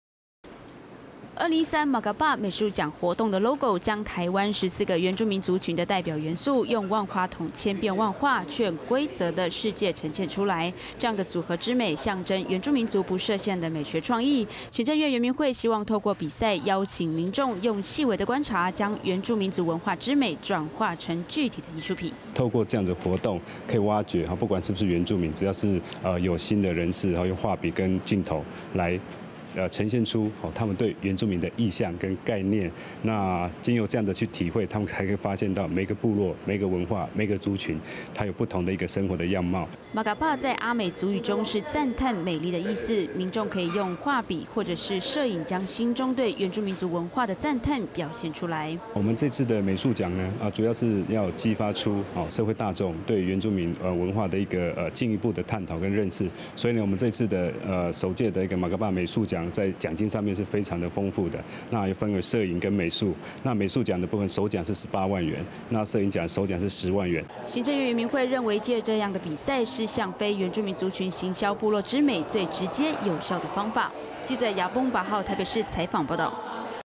Makapah美術獎 攝影.繪畫徵佳作 20130806
【Yabung/巴浩】

「2013 Makapah美術獎」活動的LOGO，將台灣14個原住民族群的代表元素，用萬花筒千變萬化卻規則的視界呈現出來，這樣的組合之美，象徵原住民族不設限的美學創意。

行政院原民會希望透過比賽，邀請民眾用細微的觀察，將原住民文化之美轉化成具體的藝術品。

「Makapah」在阿美族語中是「讚嘆、美麗」的意思，民眾可以用畫筆或是攝影將心中對原住民族文化的讚嘆表現出來。

行政院原民會認為藉這樣的比賽，是向非原住民族群行銷部落之美，最直接有效的方法。